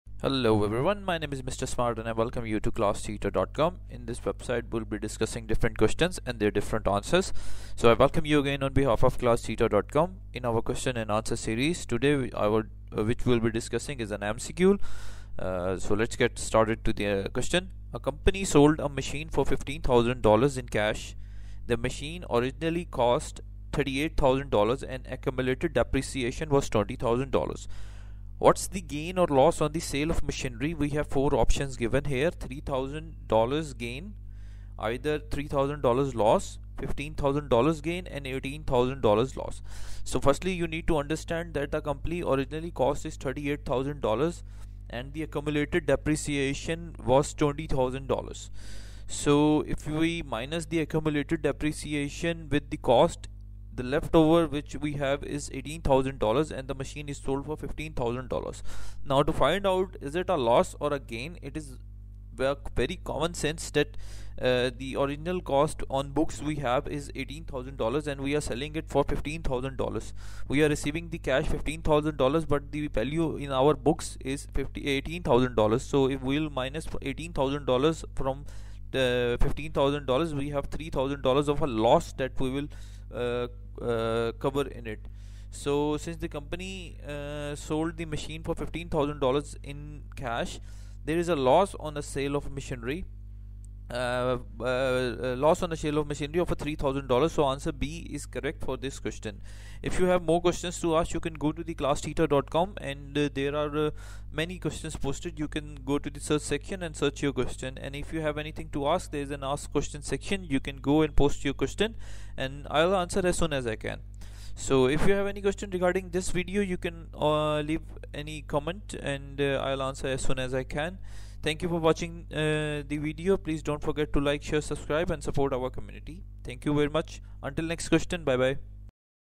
A company sold a machine for $15,000 in cash
A company sold a machine for $15,000 in cash. The machine originally cost $38,000 and accumulated depreciation was $20,000. What’s the gain or loss on the sale of machinery?
$3,000 gain
$3,000 loss
$15,000 gain
$18,000 loss